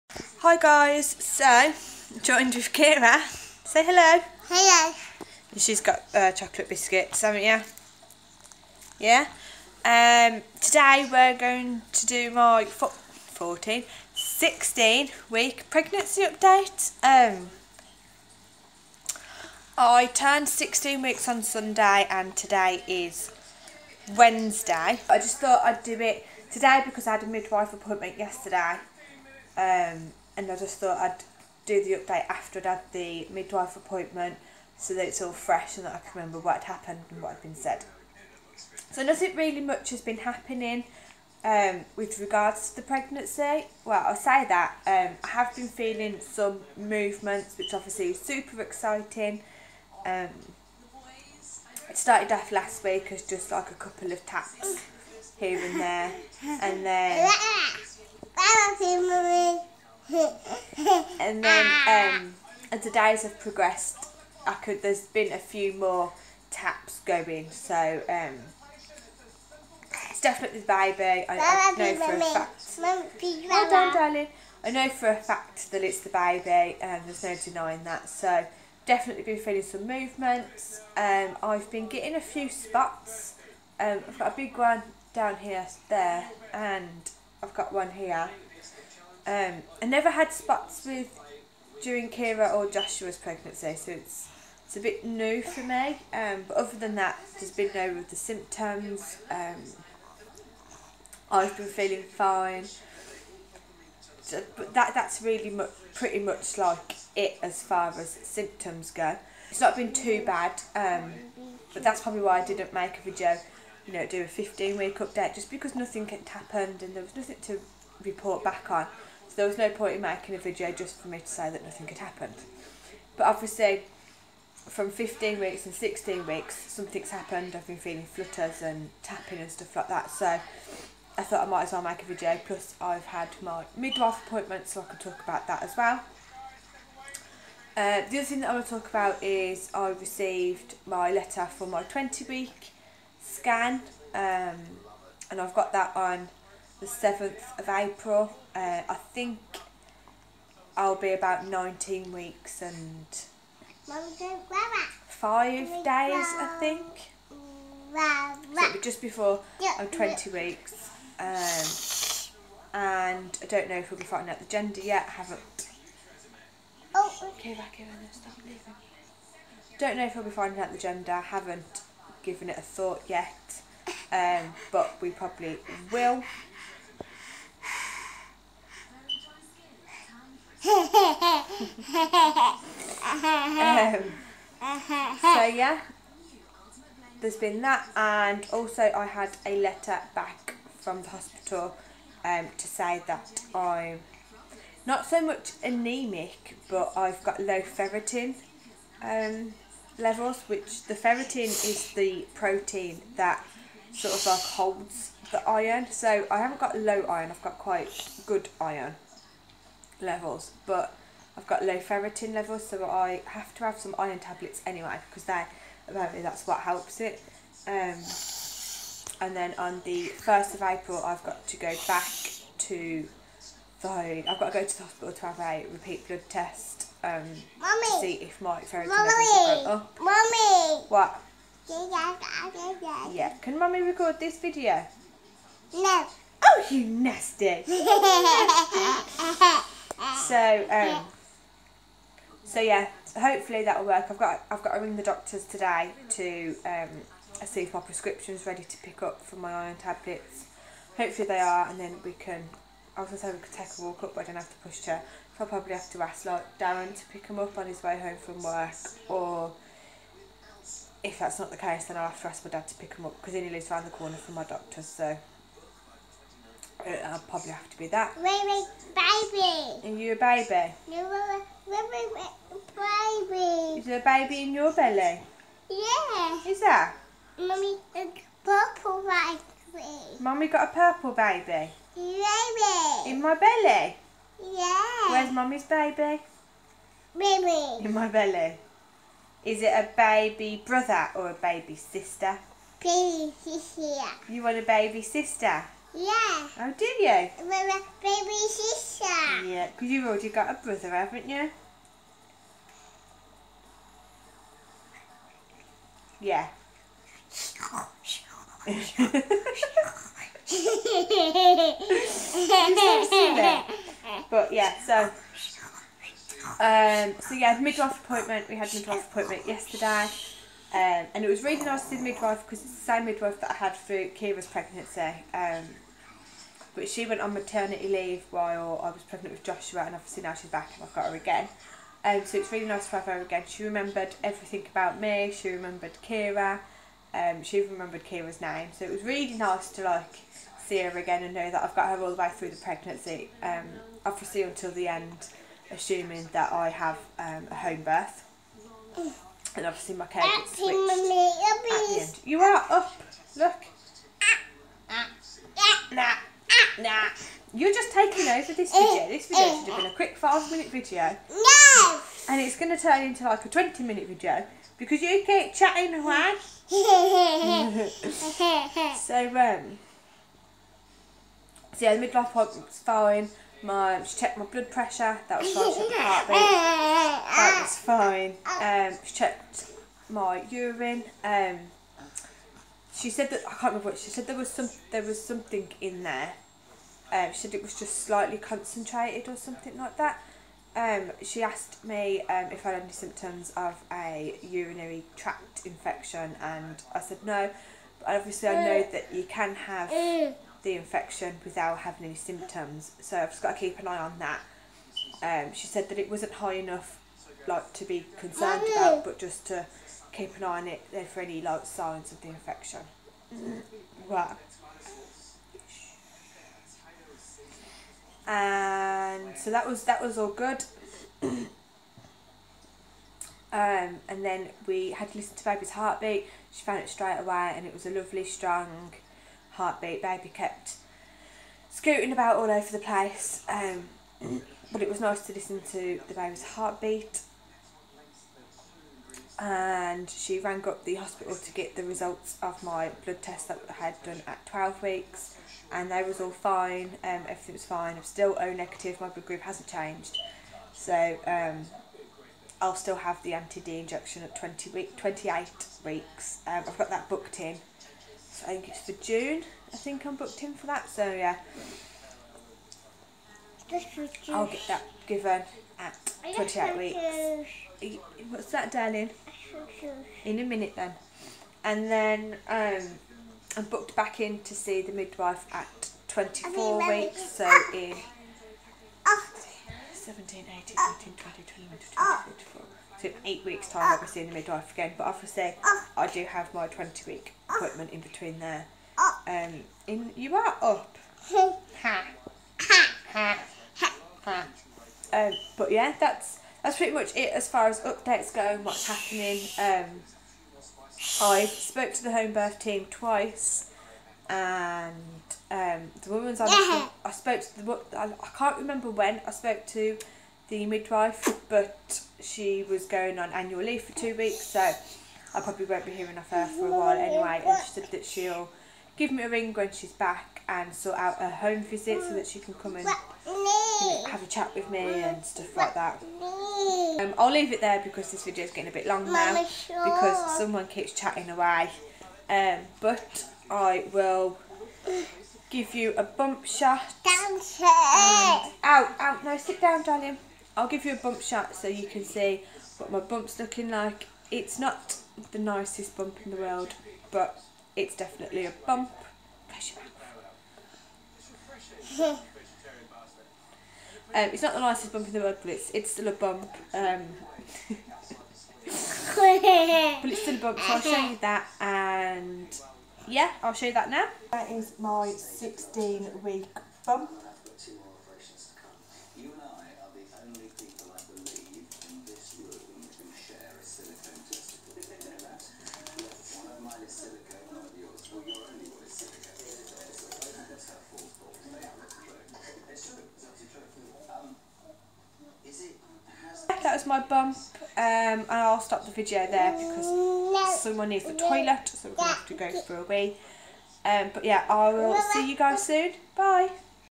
16 Week Pregnancy Update | BABY #3 - MIDWIFE APPOINTMENT
SHOW MORE!!!

I apologise for the lighting, I was sofa bound with a sleepy Joshua and a cheeky Keira! But wow, 16 weeks already. This pregnancy is going super fast already. From feeling tiny flutters to my recent midwife appointment I'll be discussing it all!

❤Amy